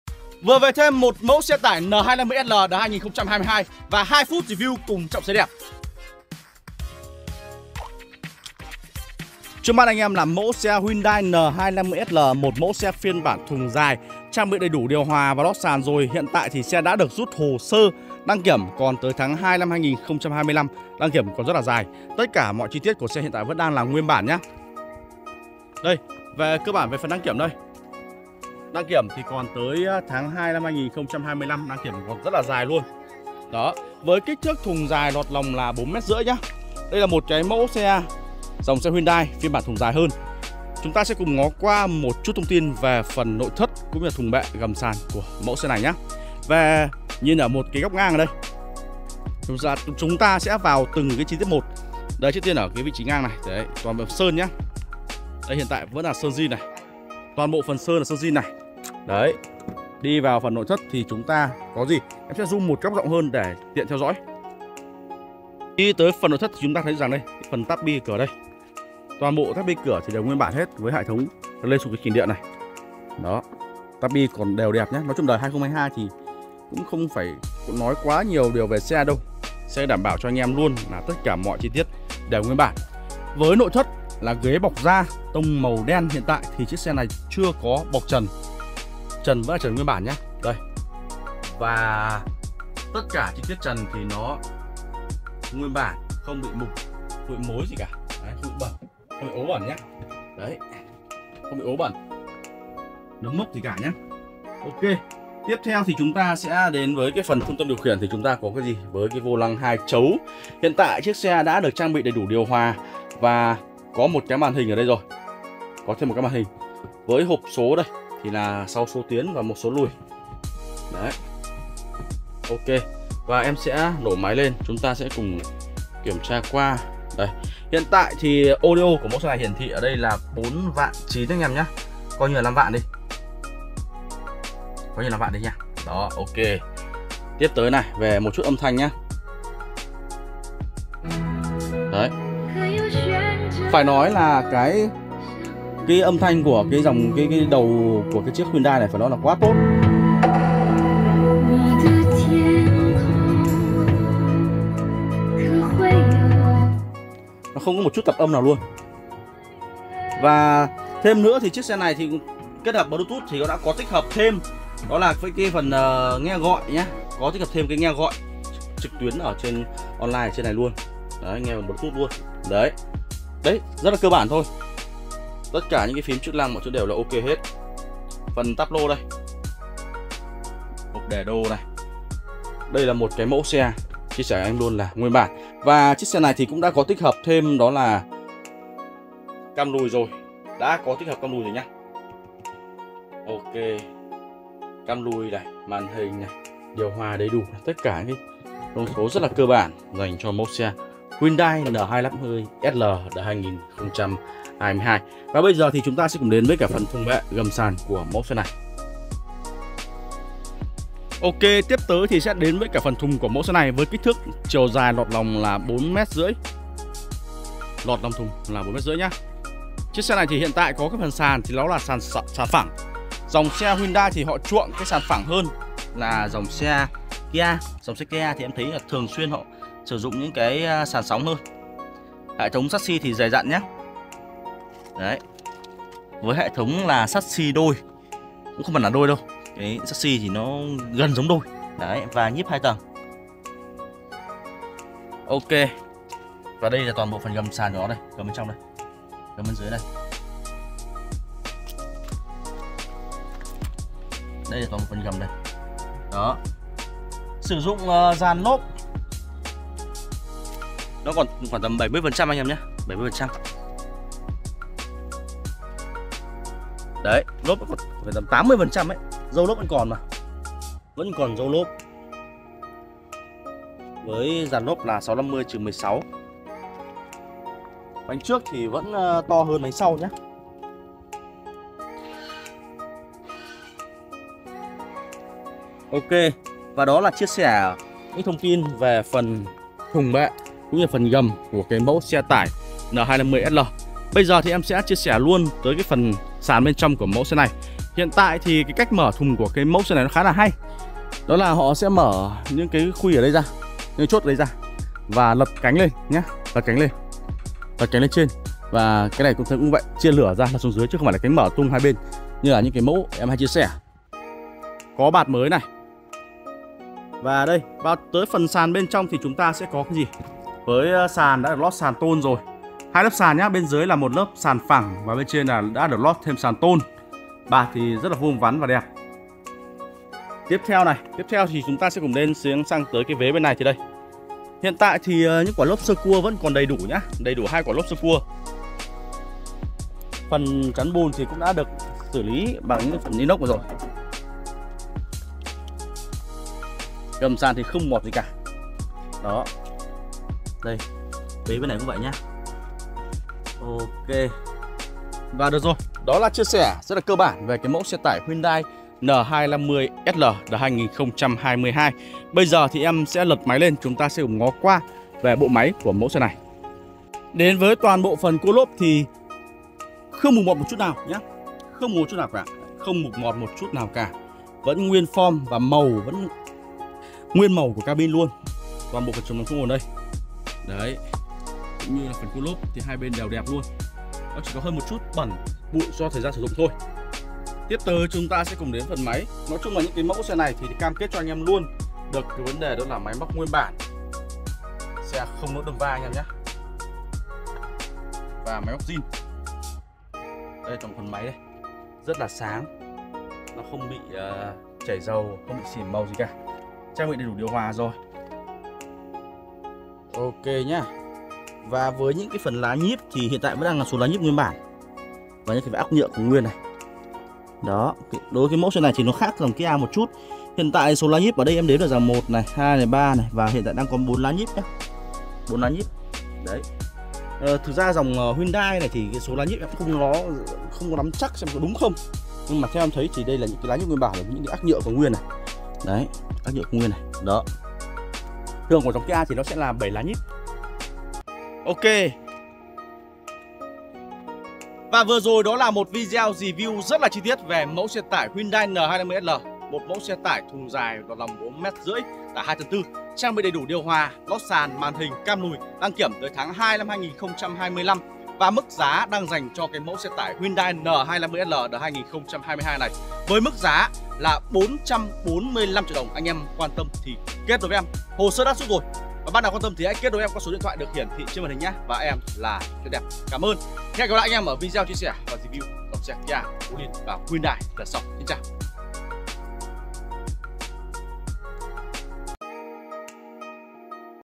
15.03.2024: Review 2' | phần 43 : Hyundai N250SL đời 2022Tải 2T4 - thùng dài lọt lòng 4m5
✨✨✨ Review 2' |
phần 43 : Hyundai N250SL đời 2022
Tải 2T4 - thùng dài lọt lòng 4m5
Đăng kiểm dài 02/25
Mời các bác tham khảo
xem xe tại thị trấn giang tiên huyện Phú Lương tỉnh Thái Nguyên
Ảnh chi tiết dưới phần bình luận 👇